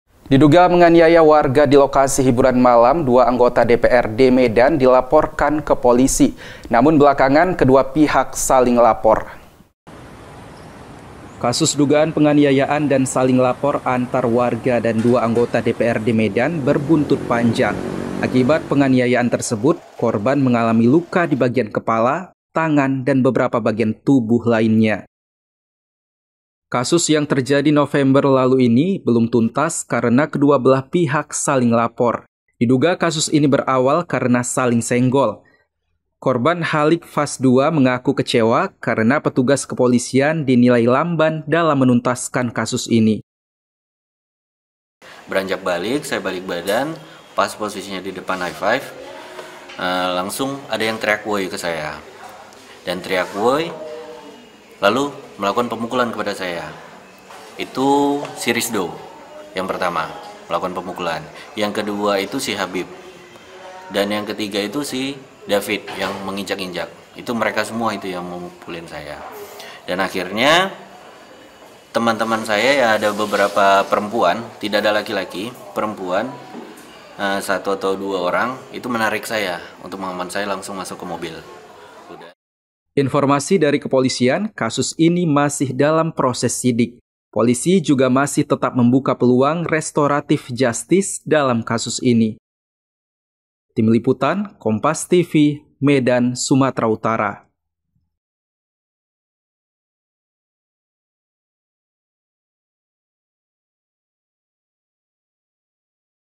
Ini Kesaksian Korban Penganiayaan di Lokasi Hiburan Malam yang Diduga Dilakukan Anggota DPRD
MEDAN, KOMPAS.TV - Diduga menganiaya warga di lokasi hiburan malam, 2 anggota DPRD Medan dilaporkan ke polisi.

Akibat penganiayaan tersebut, korban mengalami luka di beberapa bagian tubuhnya.

Kasus yang terjadi November lalu tersebut belum tuntas karena kedua belah pihak saling lapor. (*)

Kompas TV
Independen | Terpercaya